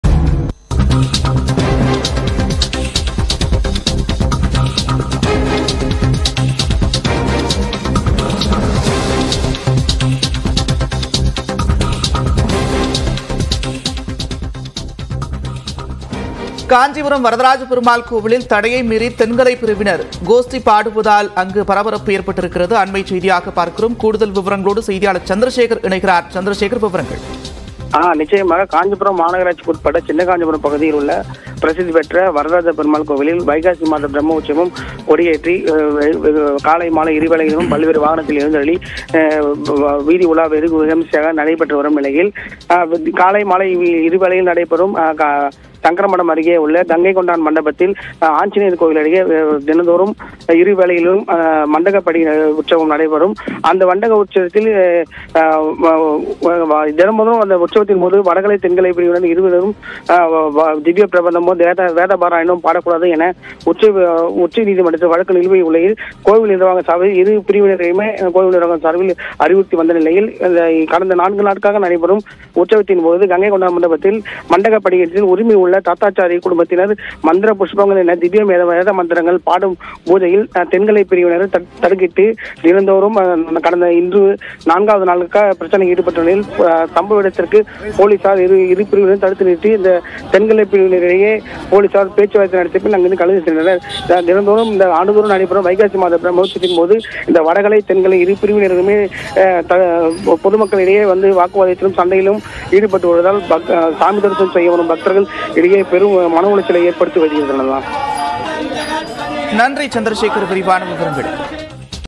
Just Now | தடையை மீறி கோஷ்டி பாடும் தென்கலை பிரிவினர் | Kanchipuram Vadakalai Thenkalai
list=PLZjYaGp8v2I8Jl8PfHo2MQdWKs42-15cX